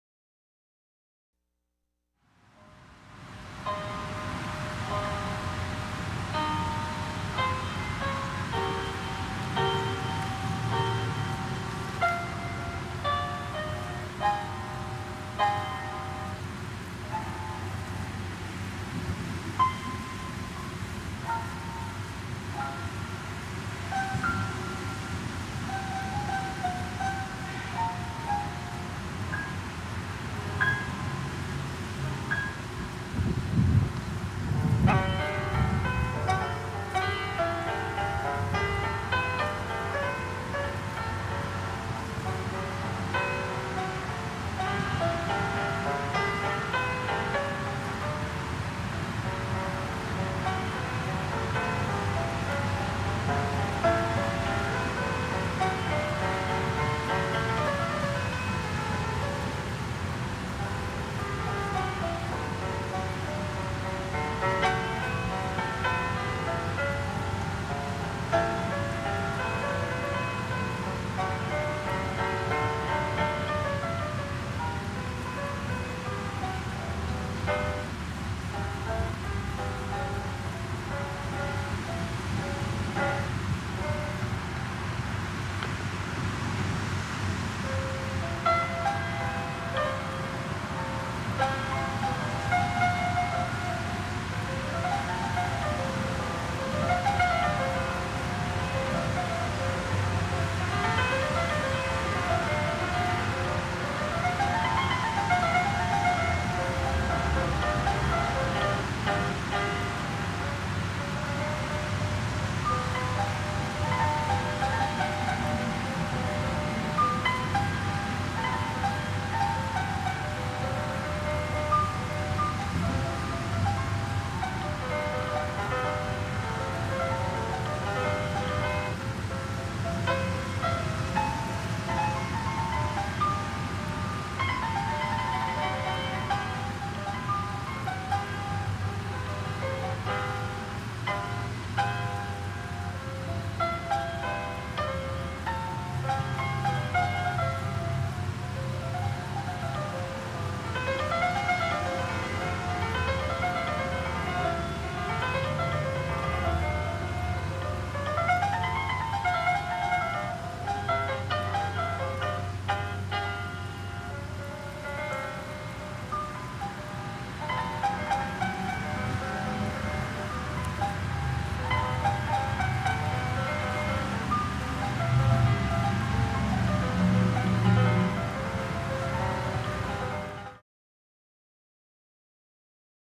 Hilarious animal grooming in time with music! (Kira Seamon:Film/piano playing )
Funny Bunny grooms to upbeat music! Charming! I brought a recording of my own piano playing outside and played it while I filmed this adorable rabbit in my garden.